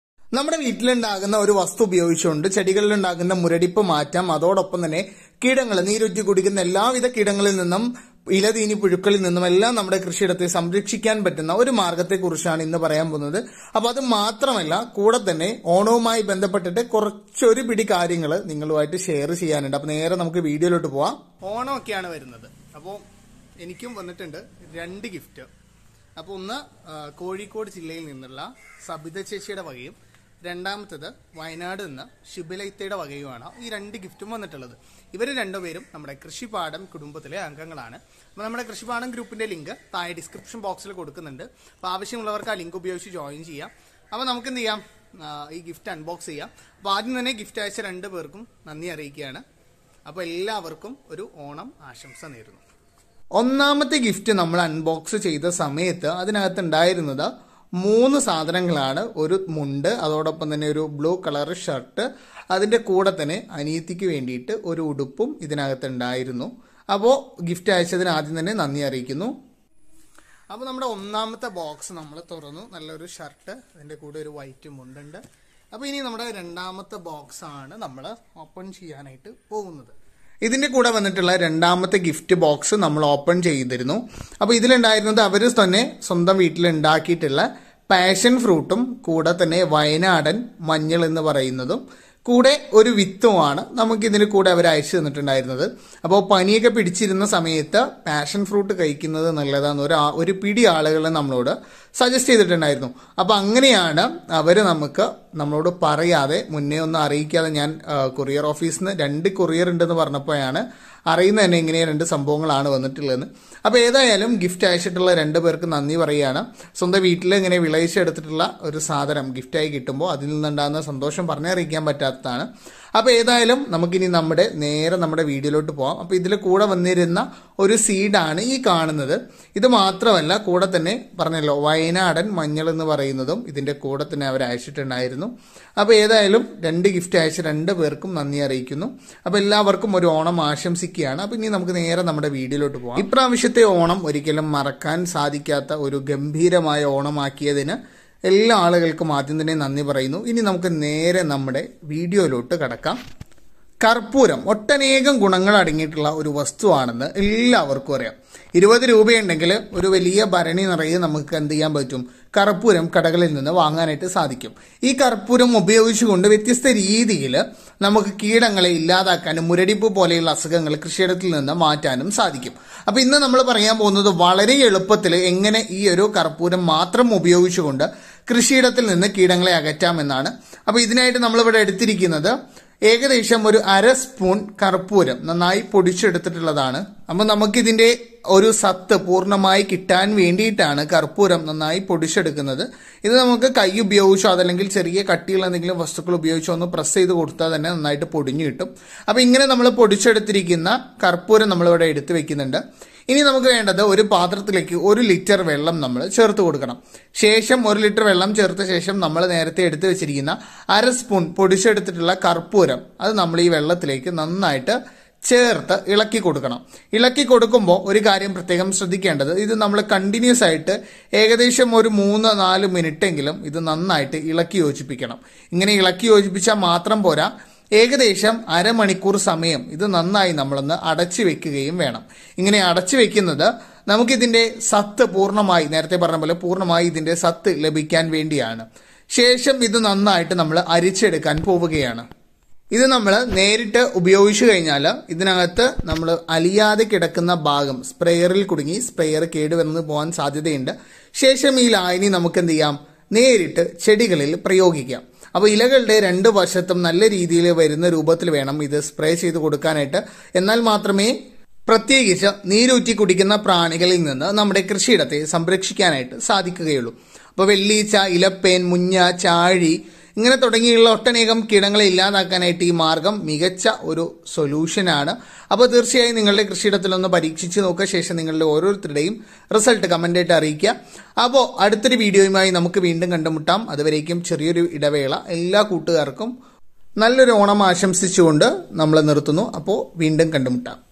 ചെടിയിലെ മുരടിപ്പ് മാറ്റാനും കീടങ്ങളെ അകറ്റാനും കർപ്പൂരം
കൃഷിപാഠം ഗ്രൂപ്പിൽ ജോയിൻ ചെയ്യുന്നതിനു താഴെയുള്ള ലിങ്കിൽ ക്ലിക്ക് ചെയ്യുക